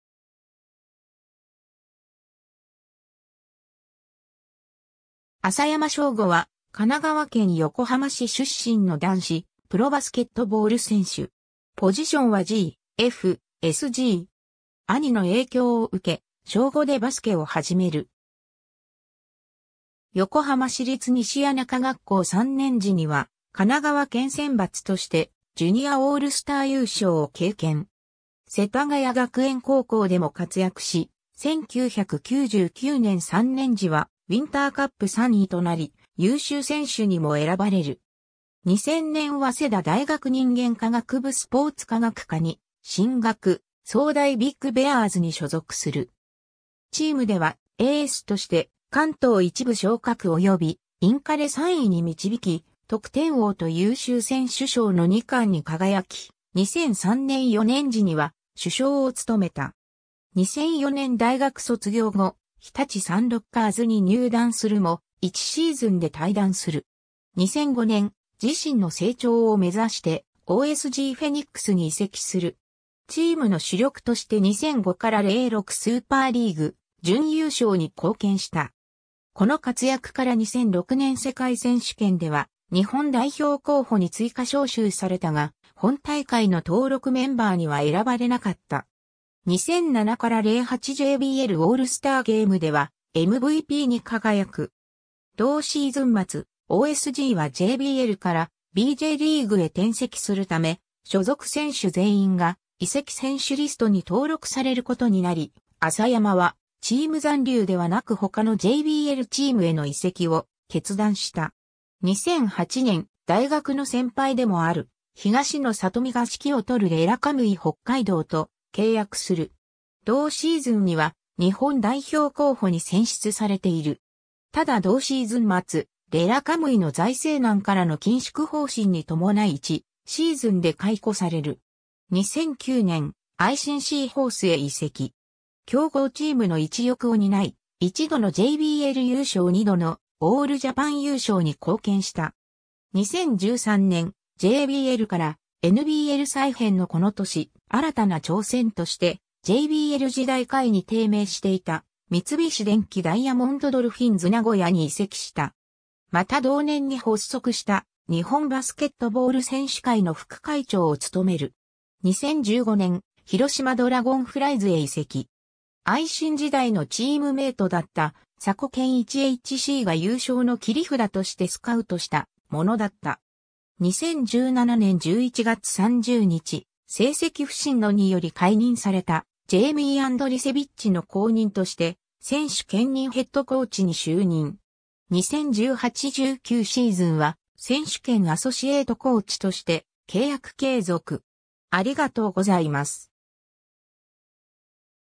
朝山正悟
Bリーグのコーチ、ゼネラルマネージャー
朝山 正悟（あさやま しょうご、1981年6月1日 - ）は、神奈川県横浜市出身の男子プロバスケットボール選手。
ポジションはG/F、SG。
 兄の影響を受け、小5でバスケを始める。
横浜市立西谷中学校3年時には神奈川県選抜としてジュニアオールスター優勝を経験。
 世田谷学園高校でも活躍し、1999年3年時はウィンターカップ3位となり優秀選手にも選ばれる。
 2000年早稲田大学人間科学部スポーツ科学科に進学、早大ビッグベアーズに所属する。
チームではエースとして関東1部昇格及びインカレ3位に導き、得点王と優秀選手賞の2冠に輝き、2003年4年時には主将を務めた。
 2004年大学卒業後、日立サンロッカーズ（現サンロッカーズ渋谷）に入団するも、1シーズンで退団する。
 2005年、自身の成長を目指してOSGフェニックス（現三遠ネオフェニックス）に移籍する。
チームの主力として2005-06スーパーリーグ準優勝に貢献した。
この活躍から2006年世界選手権では日本代表候補に追加招集されたが、本大会の登録メンバーには選ばれなかった。
2007-08JBLオールスターゲームではMVPに輝く。
同シーズン末、OSGはJBLからbjリーグへ転籍するため所属選手全員が移籍選手リストに登録されることになり、朝山はチーム残留ではなく他のJBLチームへの移籍を決断した。
 2008年、大学の先輩でもある東野智弥が指揮を執るレラカムイ北海道（現レバンガ北海道）と契約する。
同シーズンには日本代表候補に選出されている。
ただ同シーズン末、レラカムイの財政難からの緊縮方針に伴い1シーズンで解雇される。
 2009年、アイシンシーホース（現シーホース三河）へ移籍。
強豪チームの一翼を担い、1度のJBL優勝（JBL 2012-13）2度のオールジャパン（2010年・2011年）優勝に貢献した。
 2013年、JBLからNBL再編のこの年、新たな挑戦としてJBL時代下位に低迷していた三菱電機ダイヤモンドドルフィンズ名古屋（現名古屋ダイヤモンドドルフィンズ）に移籍した。
また同年に発足した日本バスケットボール選手会の副会長を務める。
 2015年、広島ドラゴンフライズへ移籍。
アイシン時代のチームメイトだった佐古賢一HCが優勝の切り札としてスカウトしたものだった。
 2017年11月30日、成績不振のにより解任されたジェイミー・アンドリセビッチの後任として選手兼任ヘッドコーチに就任。
 2018‐19シーズンは選手兼アソシエイトコーチとして契約継続。